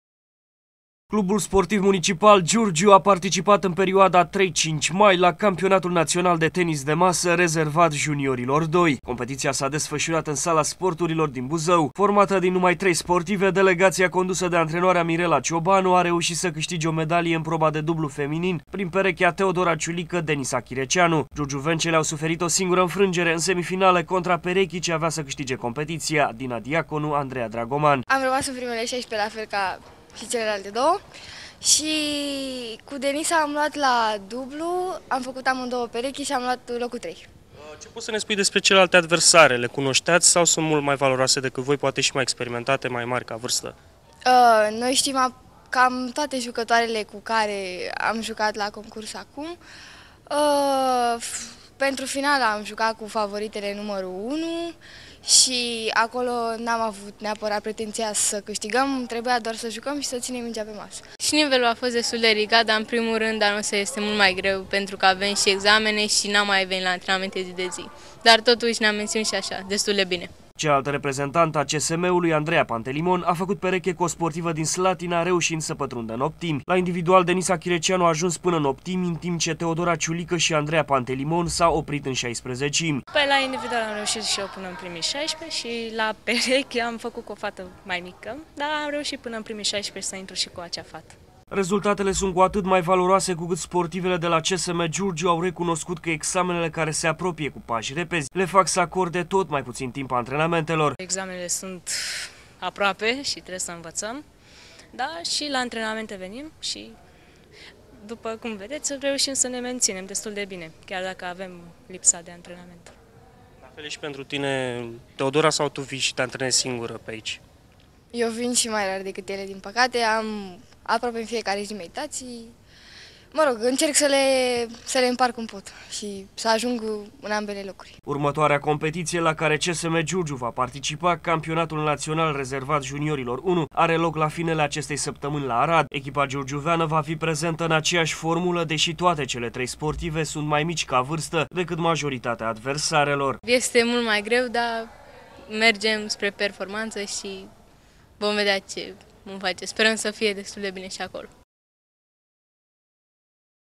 Bronz pentru tenisul de masă giurgiuvean
Secţia de tenis de masă a CSM Giurgiu a cucerit o medalie de bronz la campionatul naţional rezervat juniorilor doi în proba de dublu feminin. Perechea Teodora Ciulica -- Denisa Chireceanu a suferit o singură înfrângere, în semifinale, chiar împotriva favoritelor numărul 1 ale competiţiei.